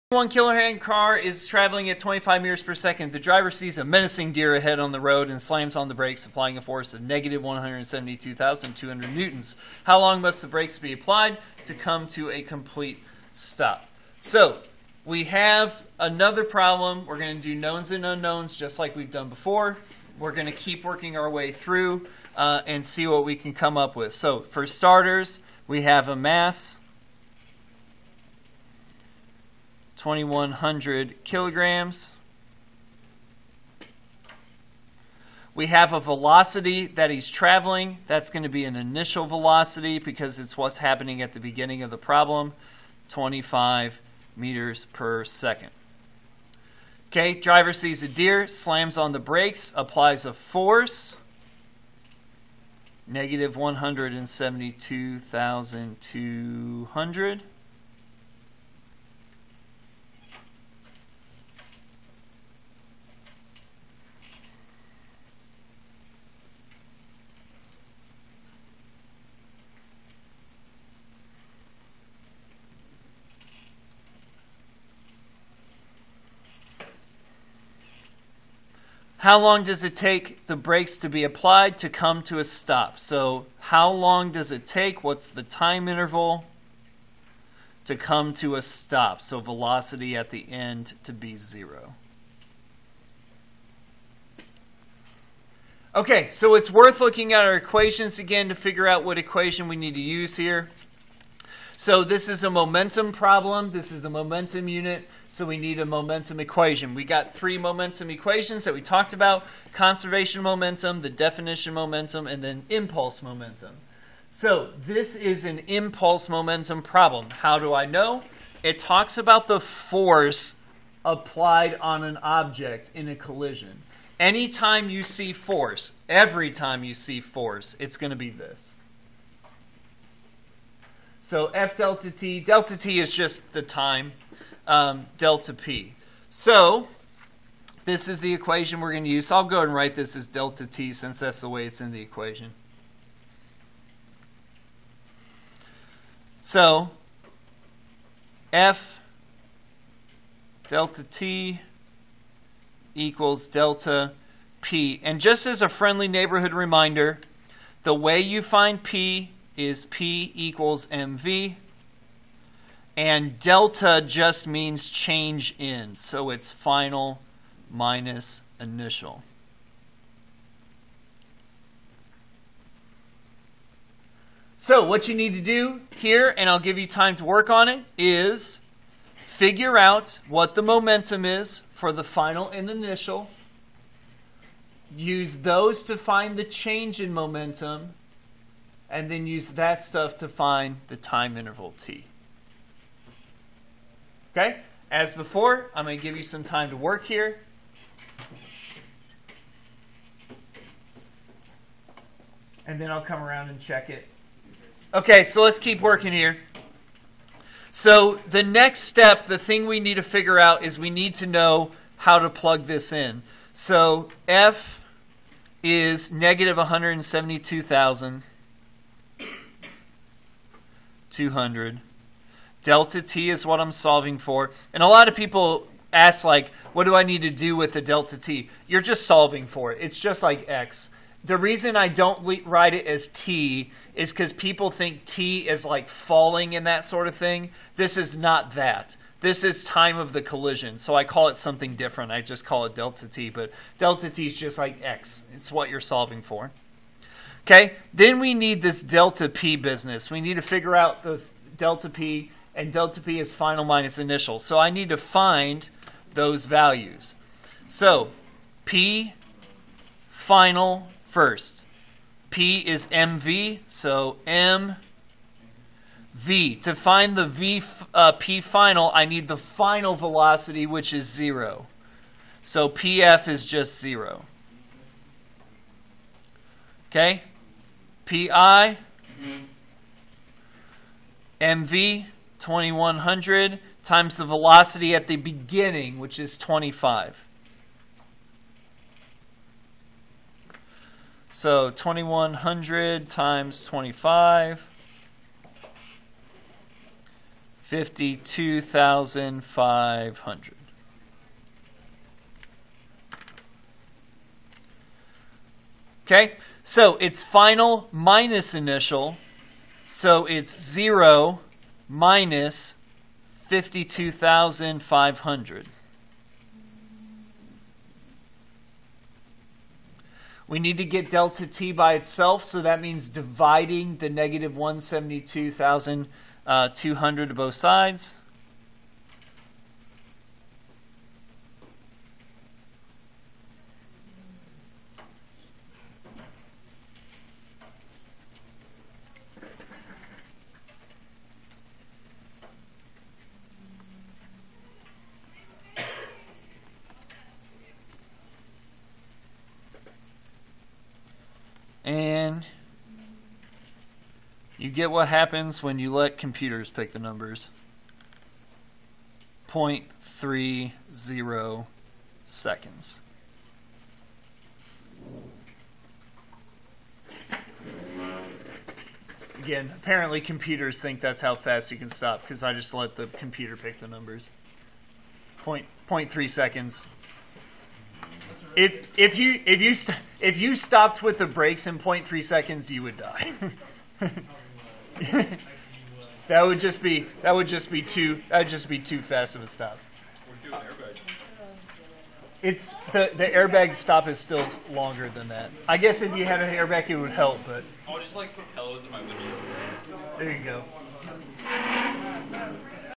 Physics 1 - Final Exam Review Guide: Impulse-Momentum Problem (#10)
Mr. B shows how to solve impulse-momentum problems